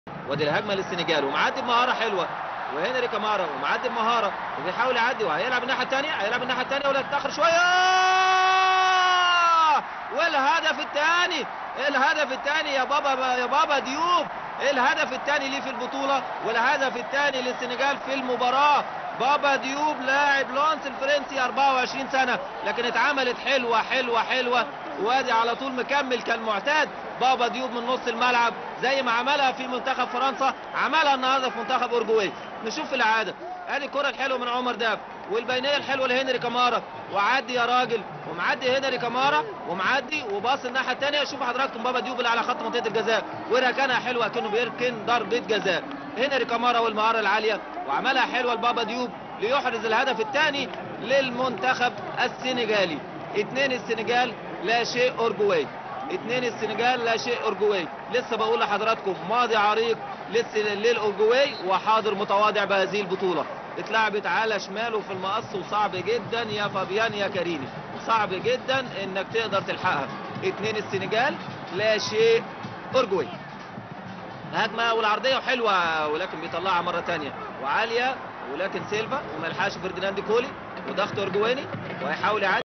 هدف السنغال الثاني في أوروجواي كأس العالم 2002 م تعليق عربي
نهائيات كأس العالم 2002 في كوريا الجنوبية واليابان
المجموعات

  الثلاثاء 11 يونيو 2002

السنغال 3 : 3 أوروجواي

'20   خليلو فاديجا
'26   بابا بوبا ديوب
'38   بابا بوبا ديوب

'46   ريتشارد موراليس
'69   دييجو فورلان
'88   ألفارو ريكوبا ضربة جزاء